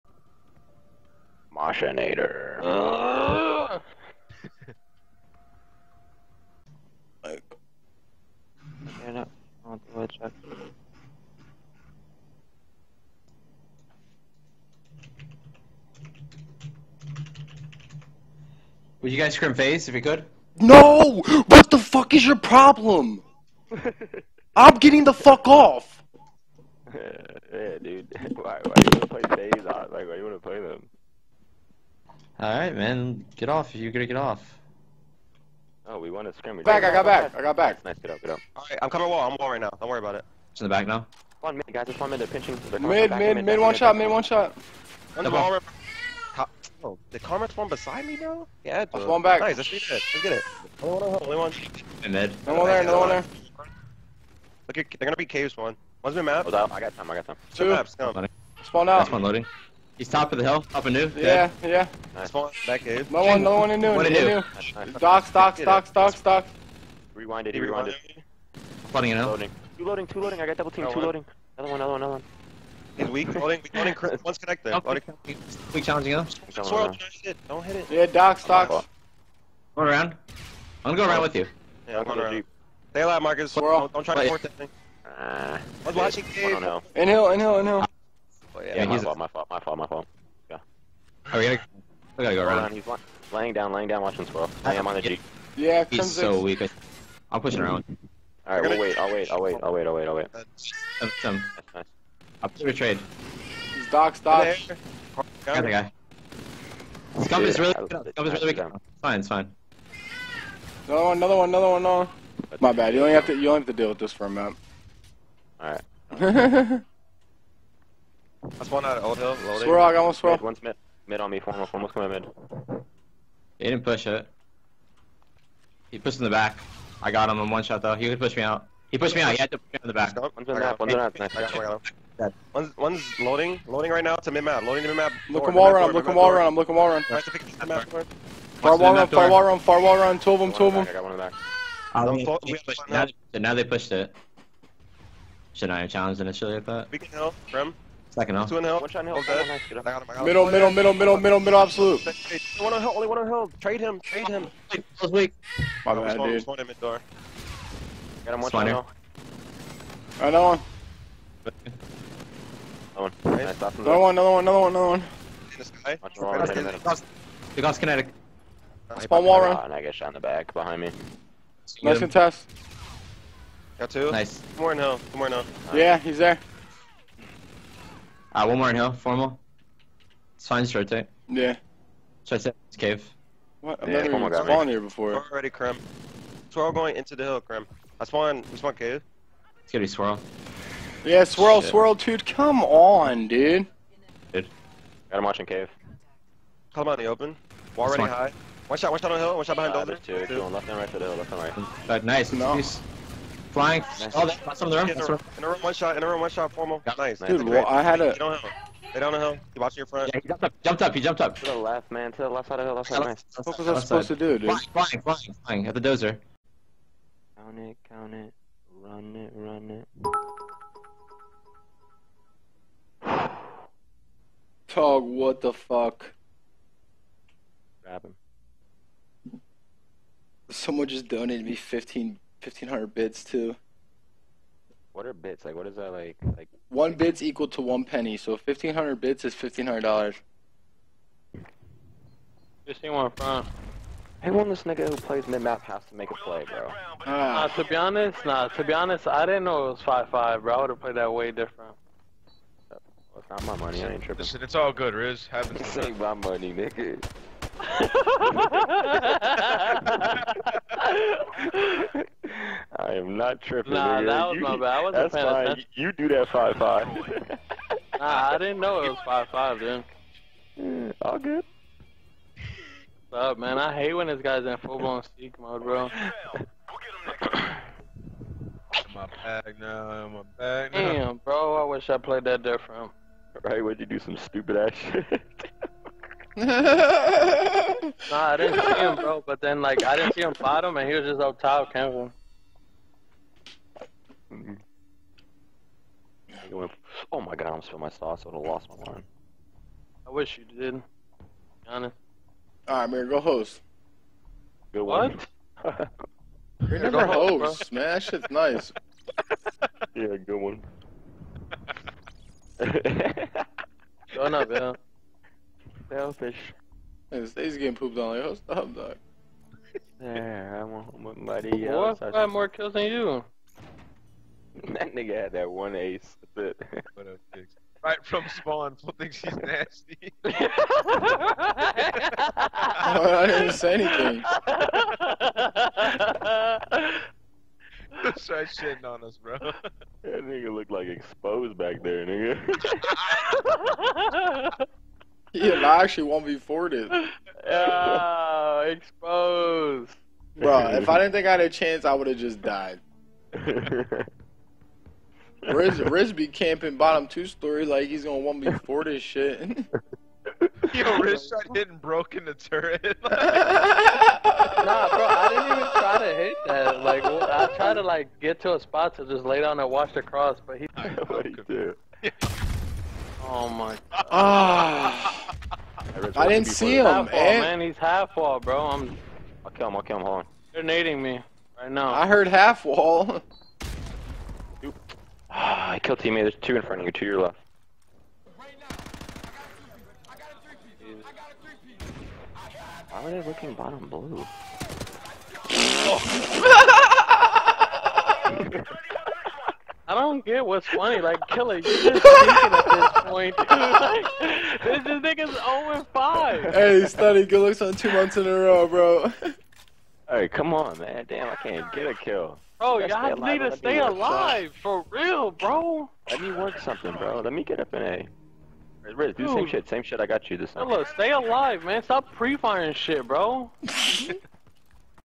KiLLa Funny Quotes/Laugh Tage (Pre Vegas)
In this episode, Killa gets into a fight with a baby, his teammates, faze, and a grenade.

Drop a like for the Bald Eagle! WOOOOOO